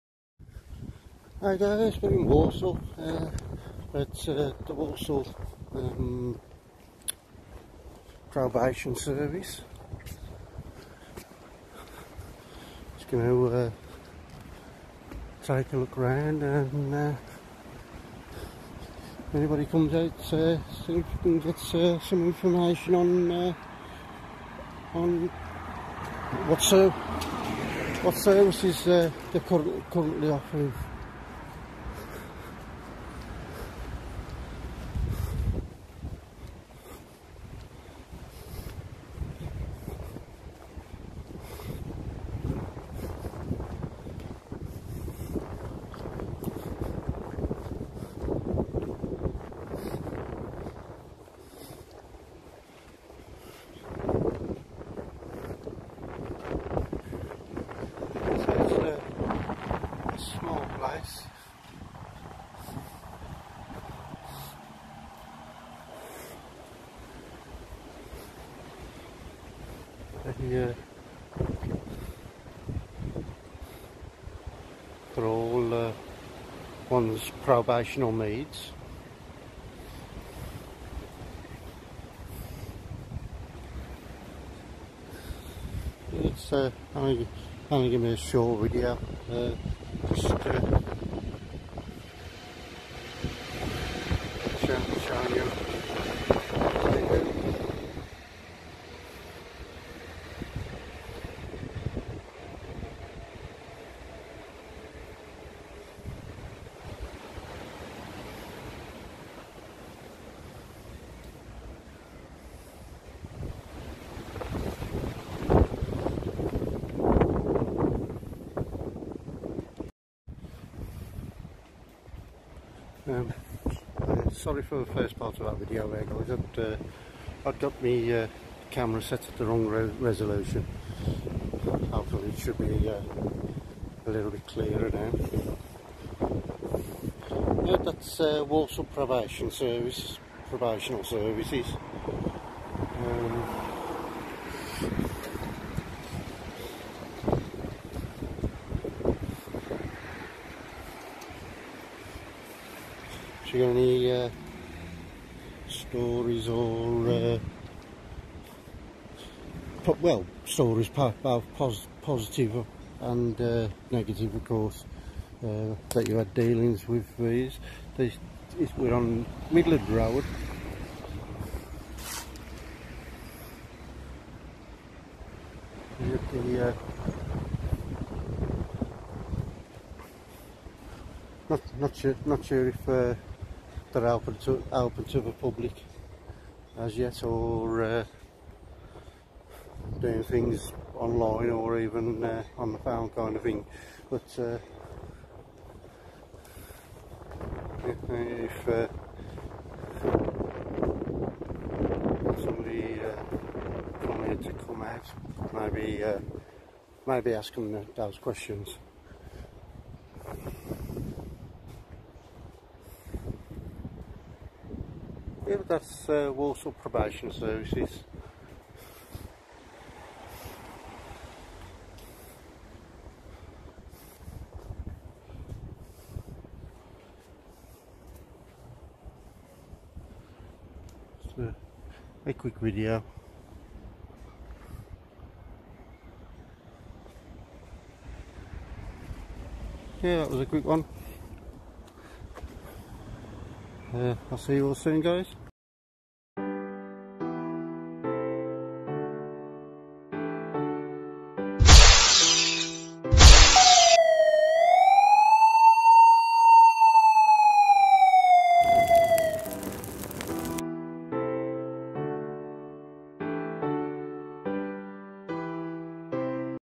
Walsall Probation Services
Went for a walk around, thought i'd share.
This is Walsall Probation Services
Midland Road,
Walsall Probation Complex,
Walsall
WS1 3QE